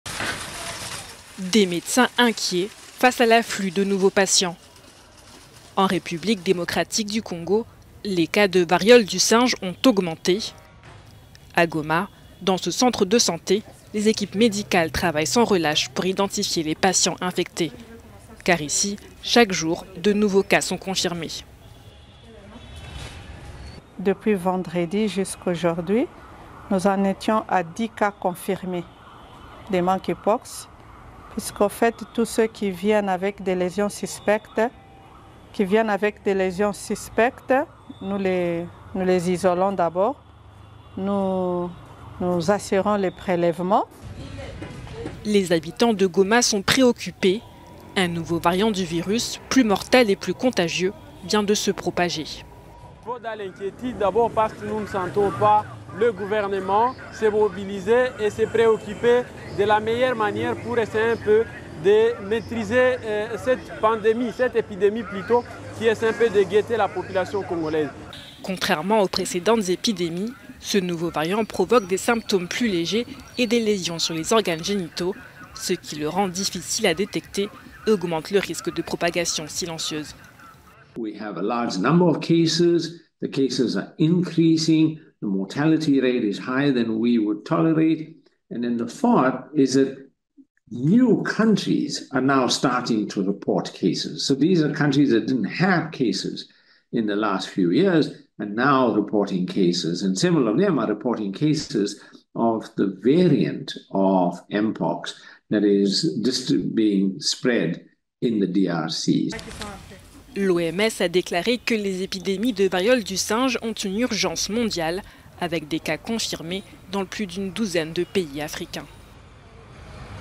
Mpox : l'OMS déclare "l'urgence mondiale", branle-bas de combat en RDC
L'OMS a déclaré une urgence mondiale suite à l'augmentation rapide des cas de variole du singe en RDC et en Afrique. Le virus, y compris un variant plus mortel, se propage dans plus d'une douzaine de pays, touchant enfants et adultes, et suscitant de vives inquiétudes.